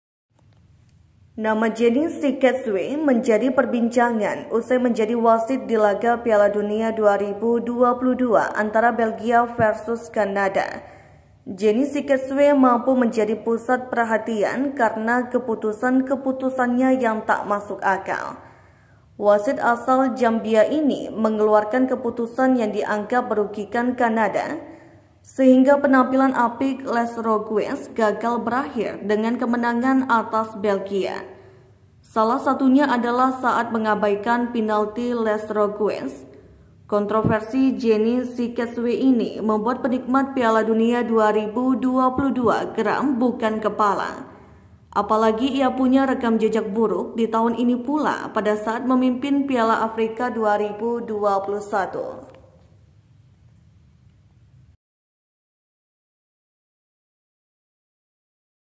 Janny Sikazwe, Wasit Belgia vs Kanada yang Penuh Kontroversi
Nama Janny Sikazwe menjadi perbincangan usai menjadi wasit di laga Piala Dunia 2022 antara Belgia vs Kanada. Janny Sikazwe mampu menjadi pusat perhatian karena keputusan-keputusannya yang tak masuk akal. Wasit asal Zambia ini mengeluarkan keputusan yang dianggap merugikan Kanada, sehingga penampilan apik Les Rogues gagal berakhir dengan kemenangan atas Belgia. Salah satunya adalah saat mengabaikan penalti Les Rogues. Kontroversi Janny Sikazwe ini membuat penikmat Piala Dunia 2022 geram bukan kepalang. Apalagi ia punya rekam jejak buruk di tahun ini pula pada saat memimpin Piala Afrika 2021.